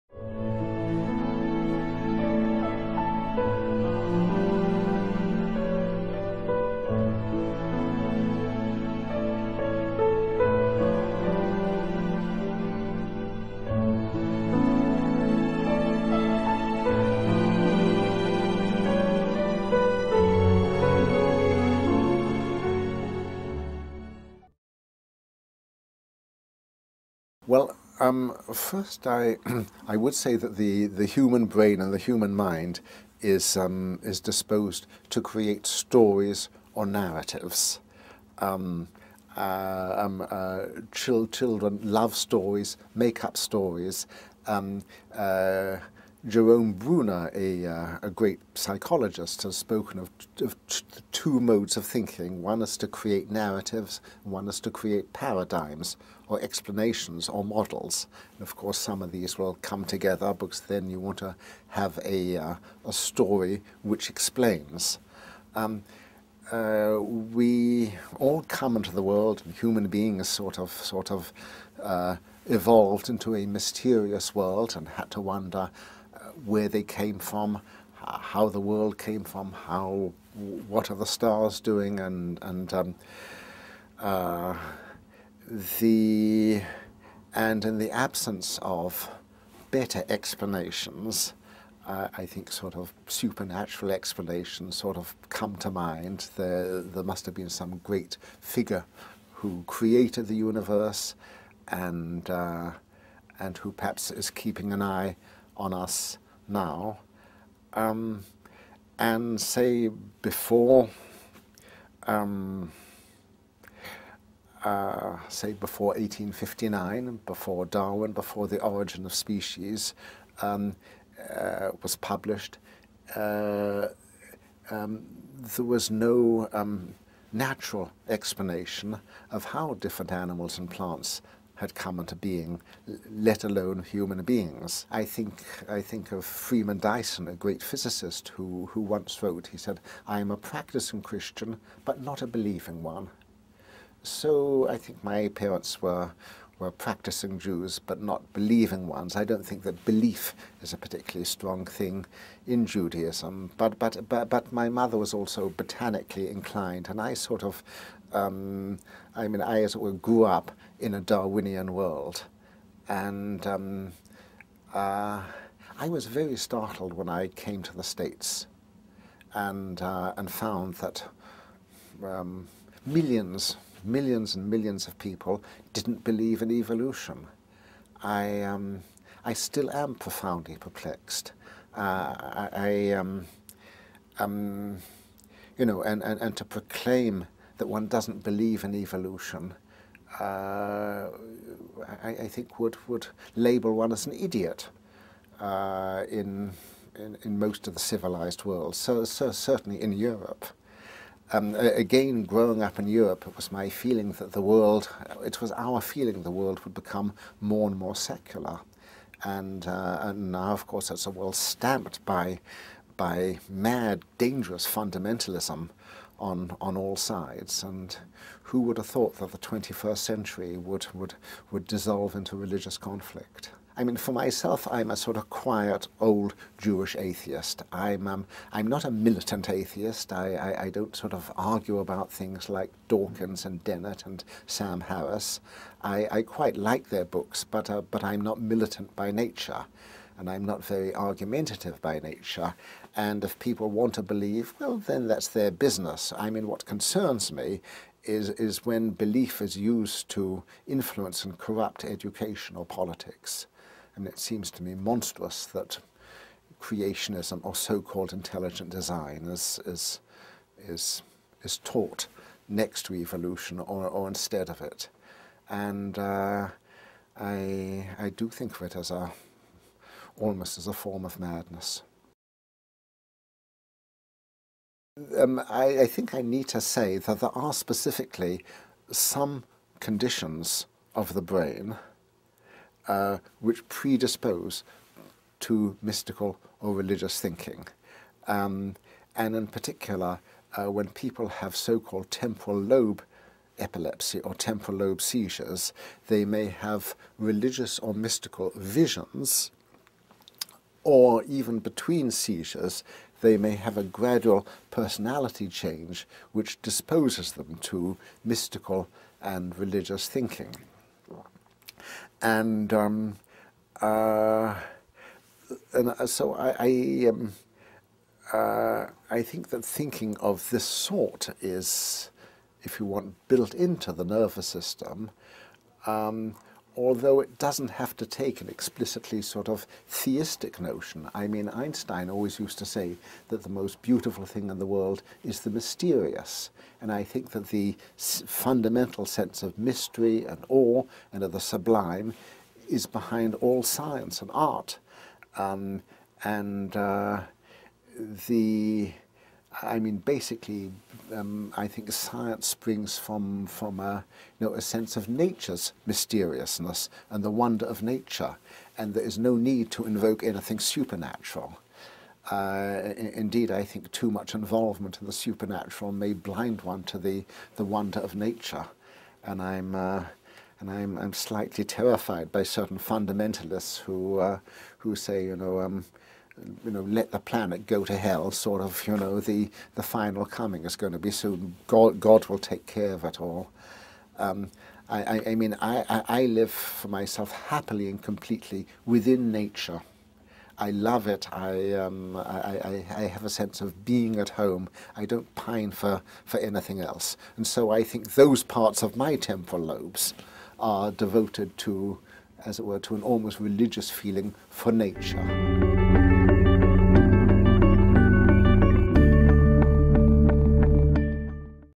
Oliver Sacks sobre el cerebro humano y la creación de mitos
El Dr. Oliver Sacks es un reconocido neurólogo, biólogo y escritor de origen británico, radicado hace varias décadas en los EE.UU.
Es el autor del libro "Awakenings" (Despertares), que fuera llevado al cine en la película homónima, protagonizada por Robert De Niro y Robin Williams (quien encarna el personaje basado en el Dr. Sacks).